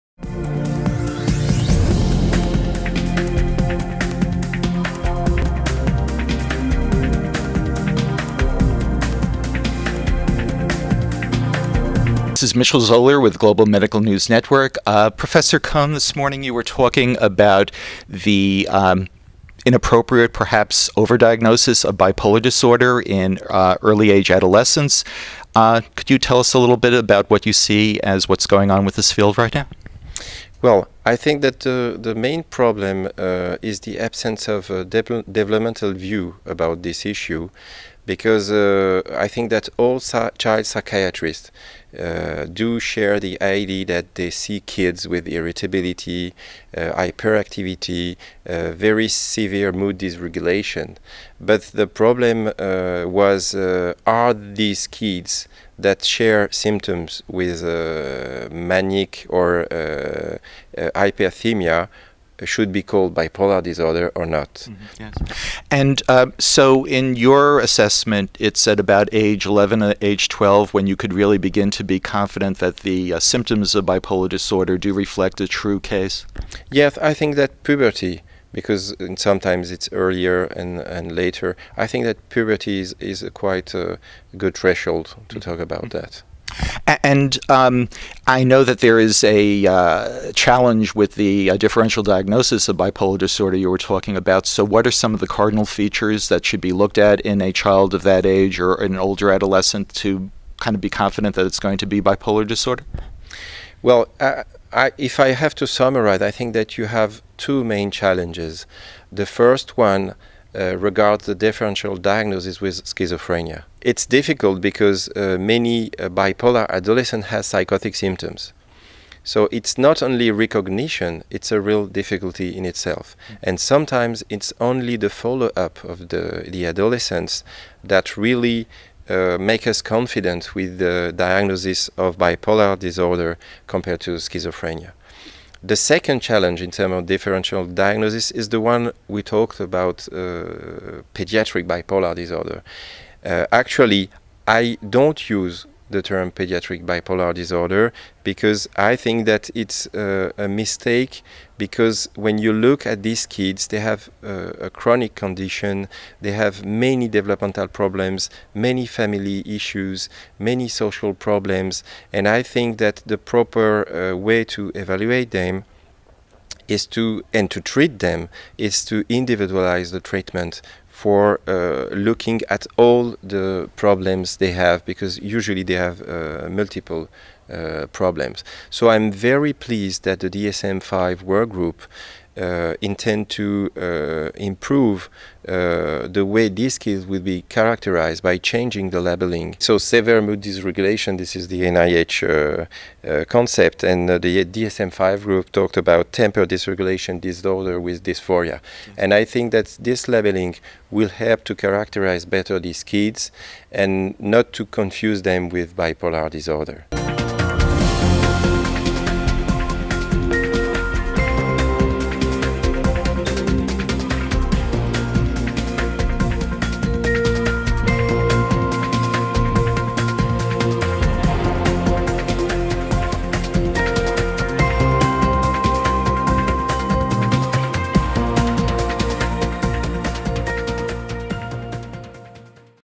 Puberty Is Good Threshold for Bipolar Diagnosis
Mitchel L. Zoler of Elsevier Global Medical News (EGMN) interviews Dr. David Cohen at the annual congress of the European College of Neuropsychopharmacology (ECNP) in Paris.